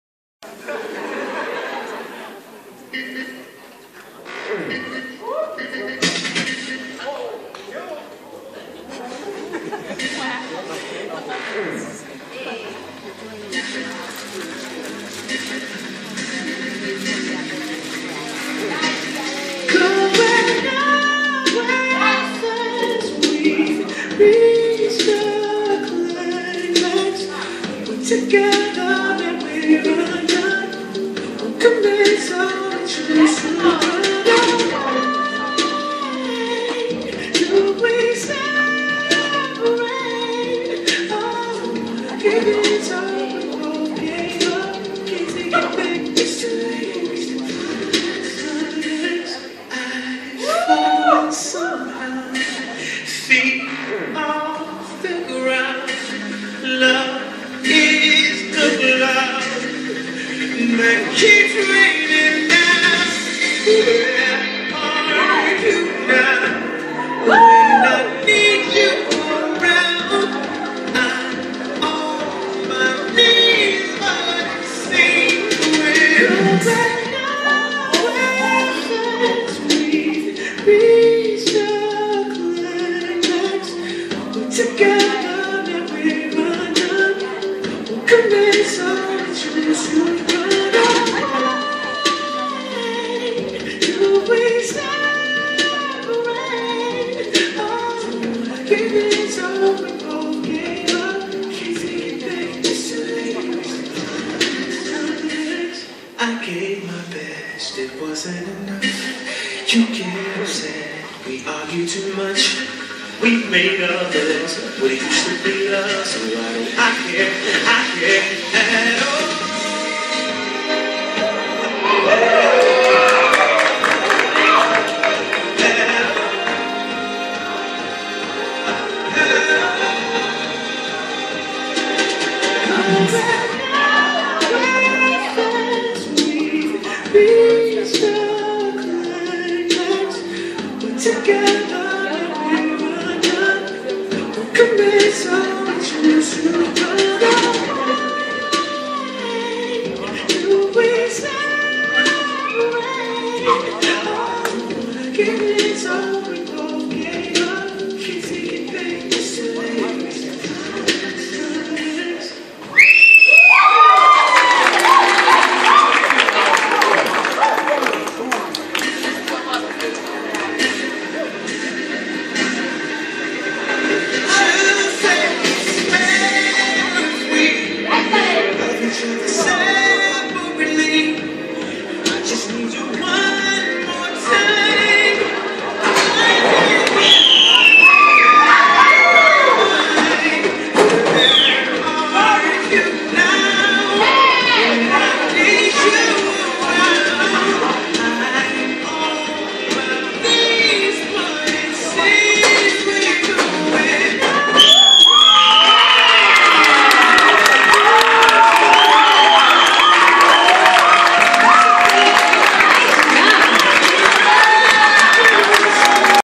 Dance Club
Fashion Show Clip: The dance club dances to usher. Enjoy! Other videos to check out: